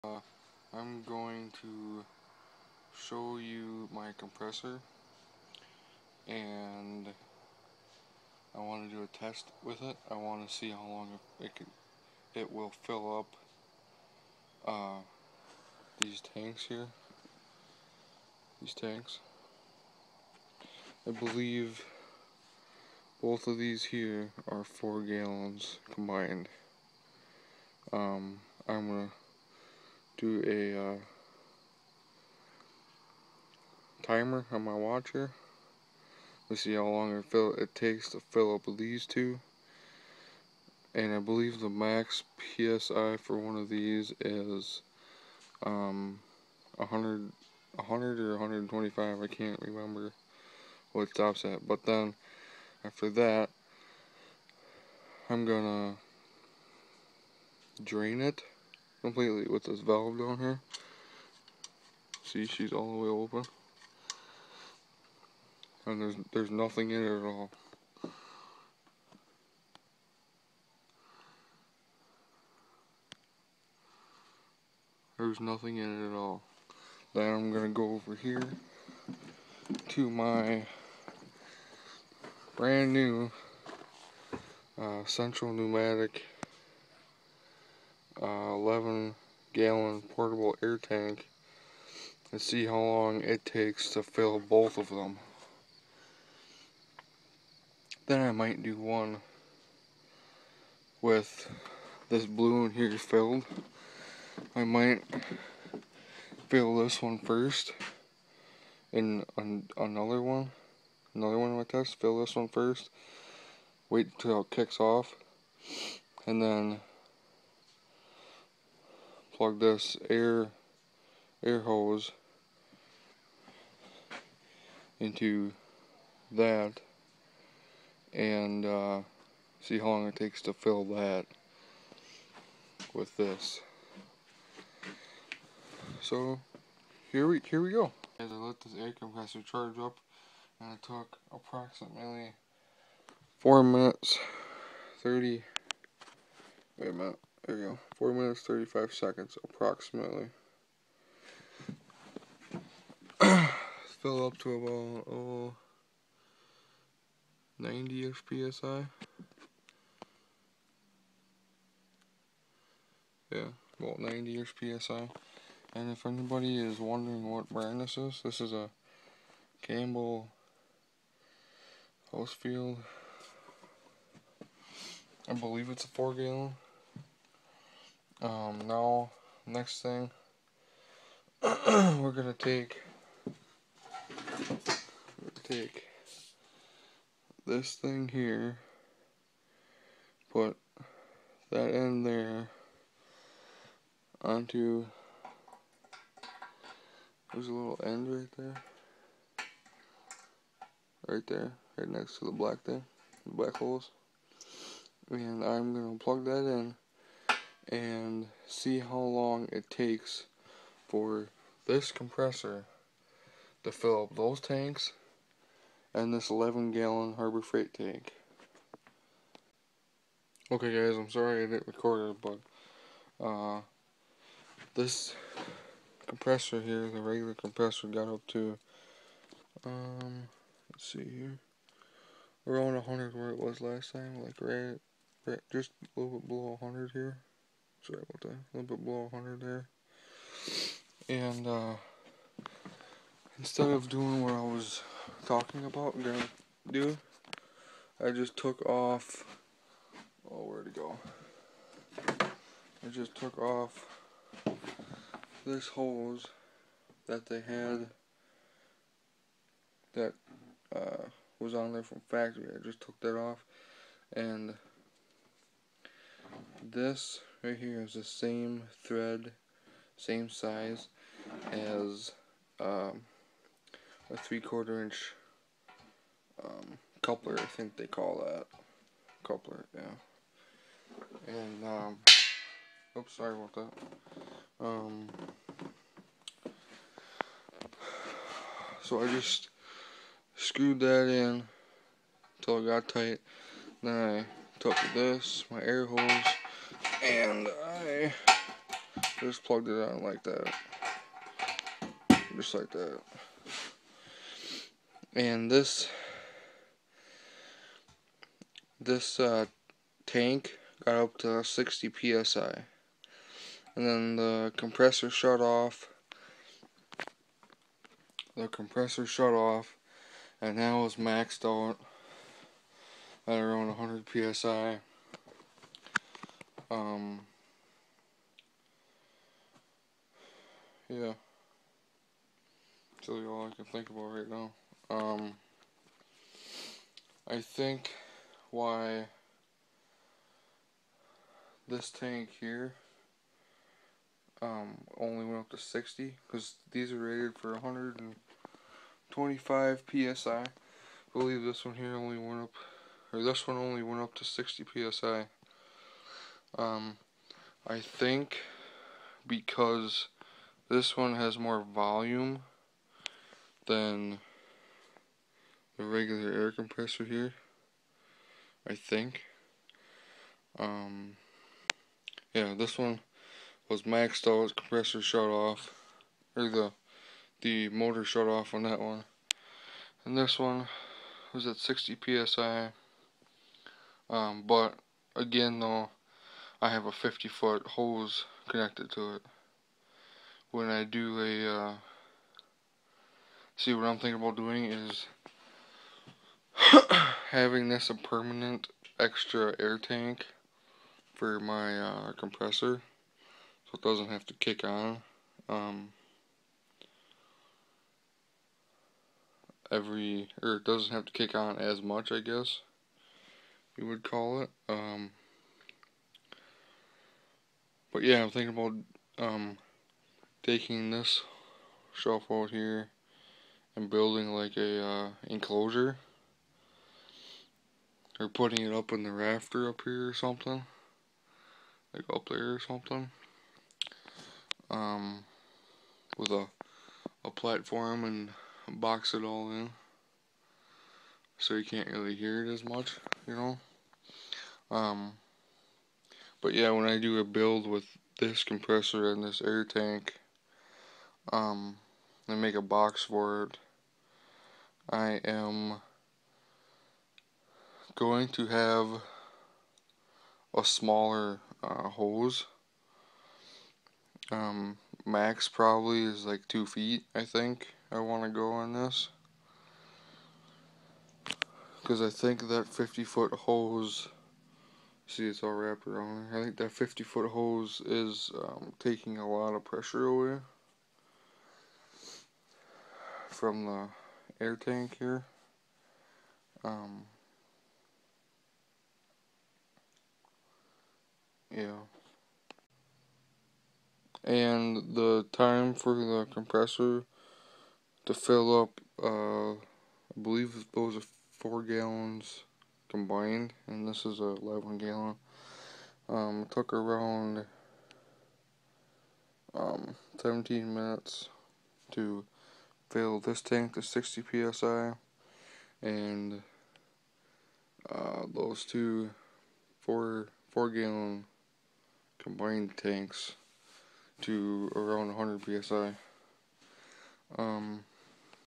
Extra air tank time testing.
Not a smaller hose but a shorter in length hose.